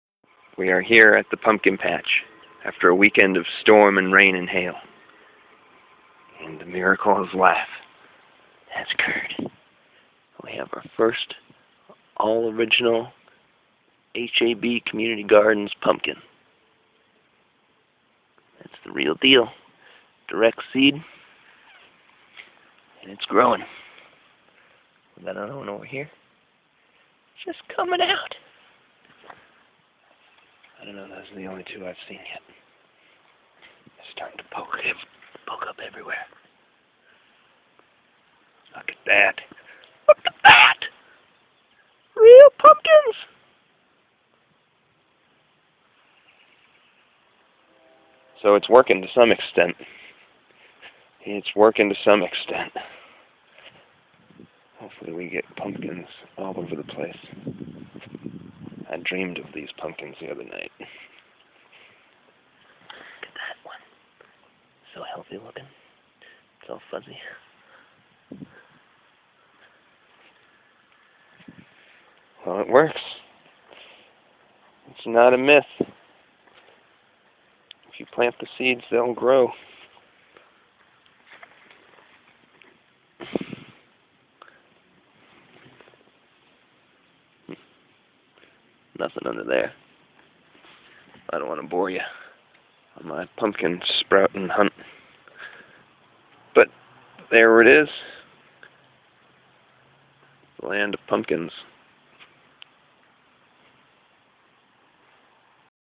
Pumpkin Patch
The Pumpkin Patch is a fundraiser and community service opportunity started in 2013 on a 50 X 50 foot plot of land. Come out this fall for story telling and pumpkin carving!!!